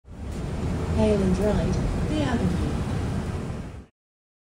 Hail & Ride the Avenue
iBus announcement recorded on route 384 (VMSS2838) on the 31/03/2024